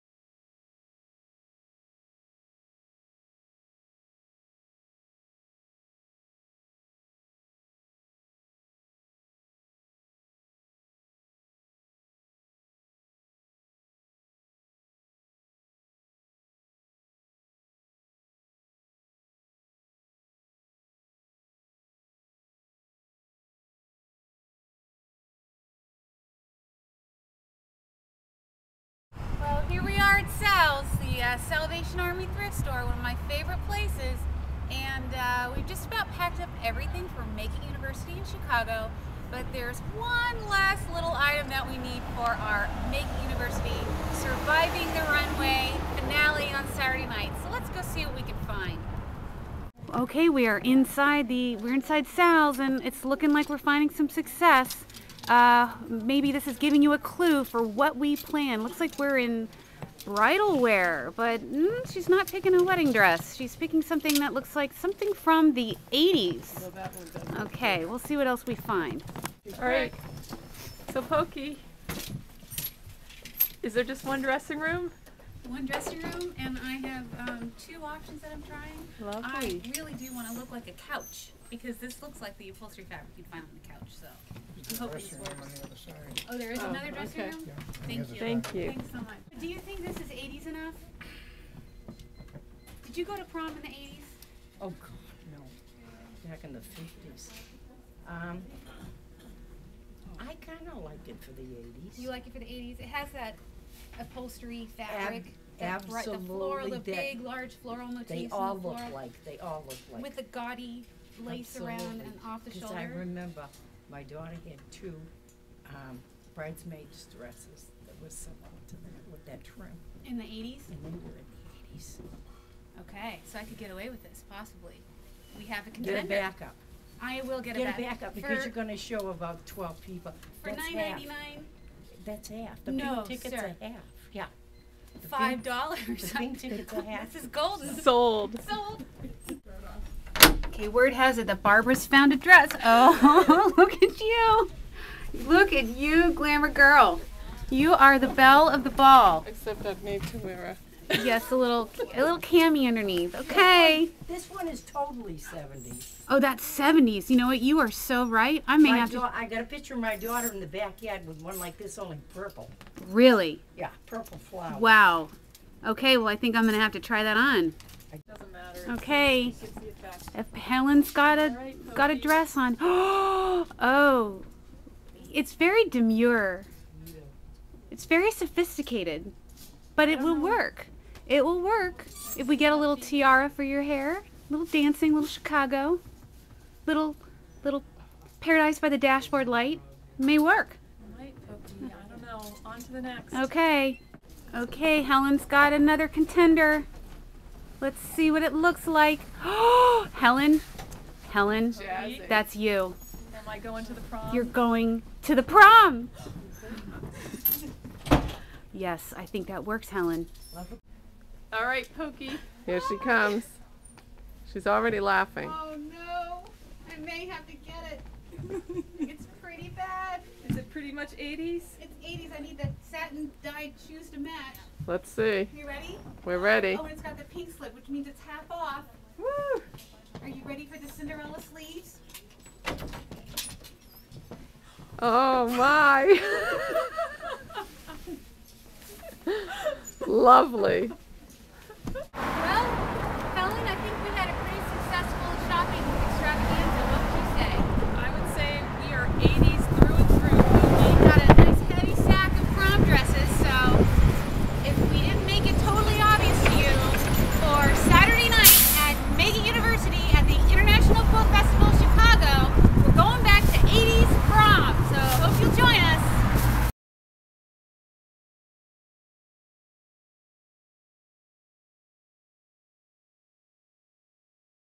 Surviving the Runway prep
A sneak peek for what we are planning this time for "Surviving the Runway"  at Make It University!(TM) in Chicago April 17 - 19, 2009. Bring your sense of creativity, your '80s flair, and join us!